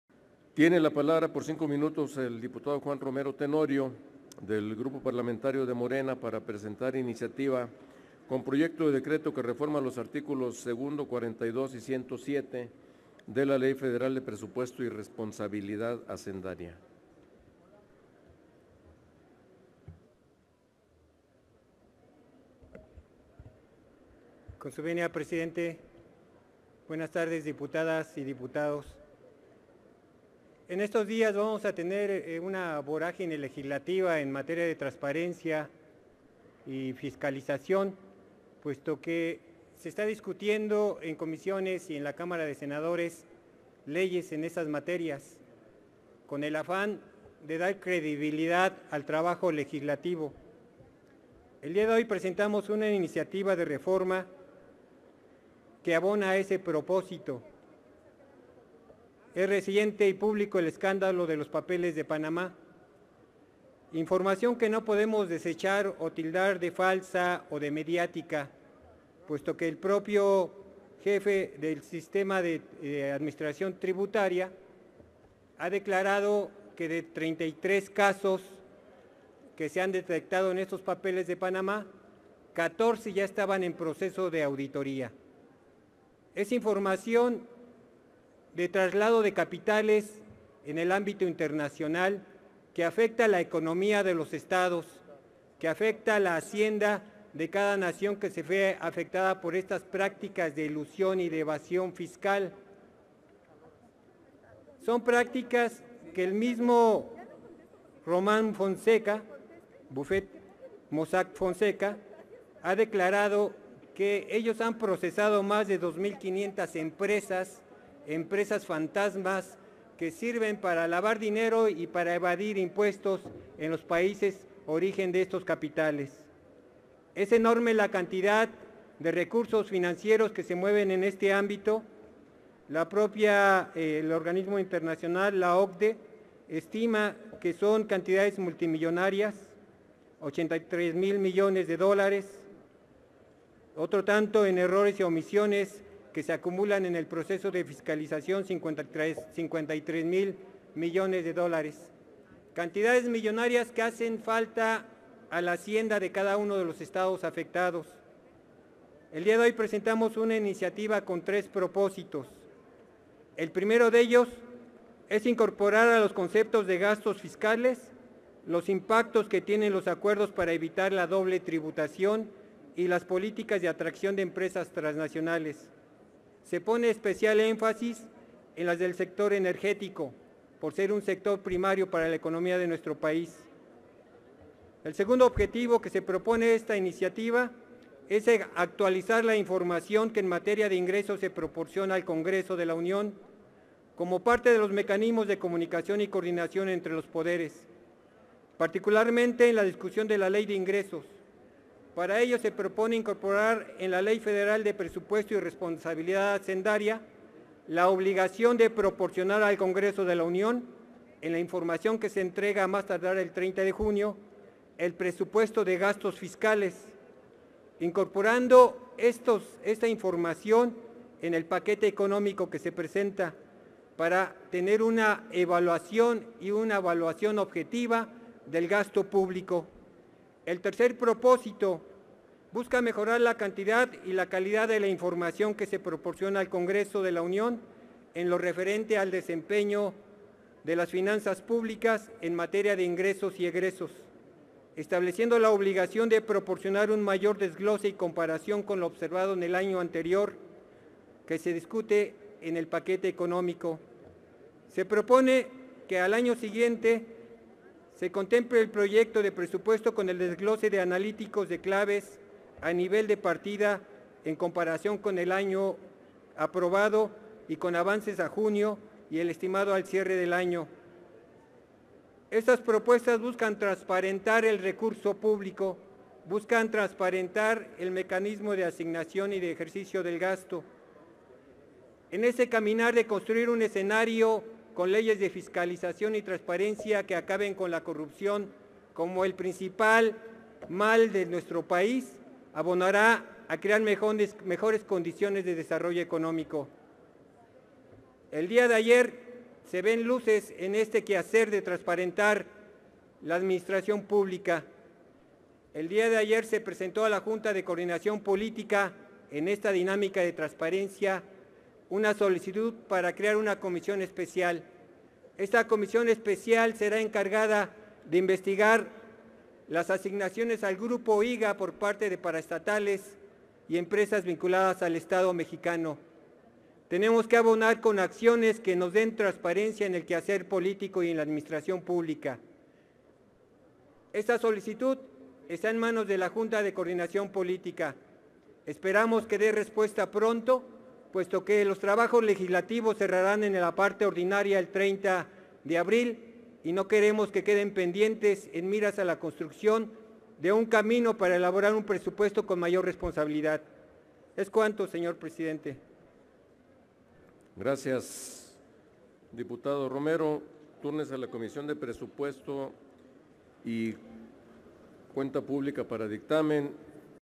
Juan Romero: pide establecer el concepto de Presupuesto de Gastos Fiscales
Juan Romero Tenorio presenta iniciativa que reforma los artículos 2°, 42 y 107 de la Ley Federal de Presupuesto y Responsabilidad Hacendaria. La iniciativa tiene por objeto establecer el concepto de Presupuesto de Gastos Fiscales.